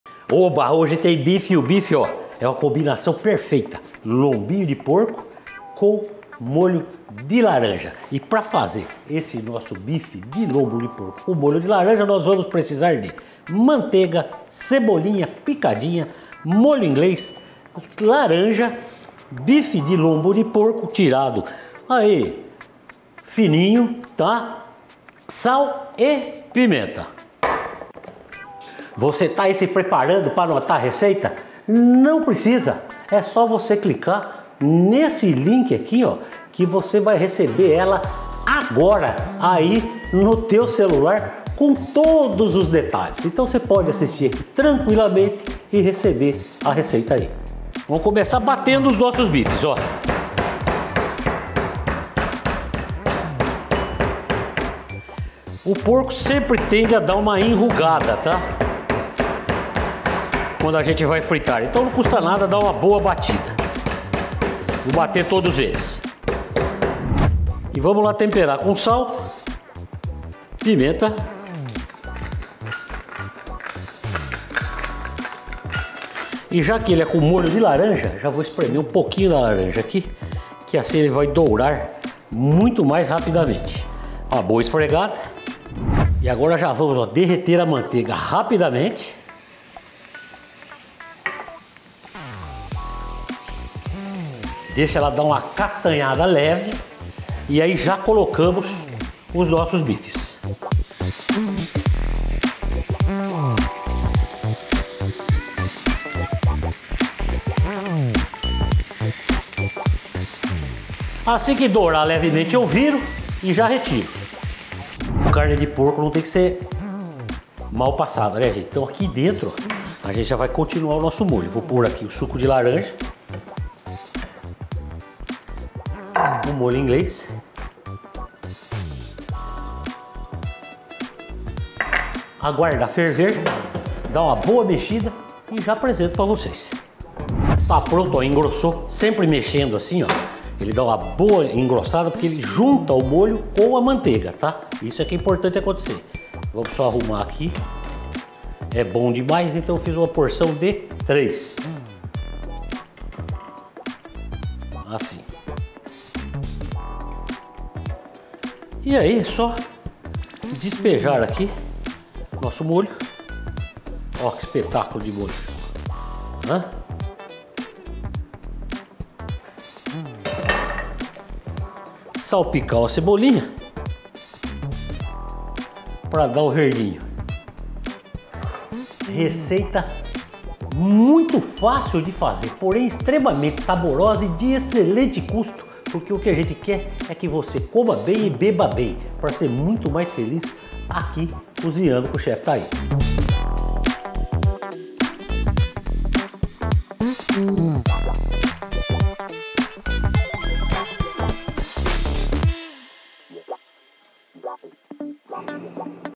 APRENDA COM O CHEF TAICO: MARAVILHOSA RECEITA DE BIFE DE LOMBO DE PORCO COM MOLHO DE LARANJA
Amigos, a receita hoje é de um saboroso Bife de Lombo de Porco com Molho de Laranja. Super fácil de preparar e fica uma delícia. Espero que gostem. Baixe a receita escrita com todos os ingredientes, é só clicar neste

Receita: Bife de Lombo de Porco com Molho de Laranja
Tempo de preparo: 20 min | Serve 02 pessoas

Ingredientes:
04 bifes de lombo de porco
01 colher de sopa de manteiga
01 colher de sopa de molho inglês
Suco de 02 laranjas
Sal e pimenta do reino
Cebolinha picadinha

Modo de preparo:
Bata os bifes com um martelo de carne.
Tempere com sal e pimenta do reino.
Adicione 01 colher de sopa do suco de laranja e esfregue bem.
Derreta a manteiga, e doure os bifes dos dois lados, mexendo sempre a frigideira.
Com os bifes na frigideira, adicione o suco de laranja e o molho inglês.
Aguarde ferver e engrossar mexendo sempre a frigideira.
Sirva os bifes com o molho, salpicando a cebolinha.

Abraços

Chef Taico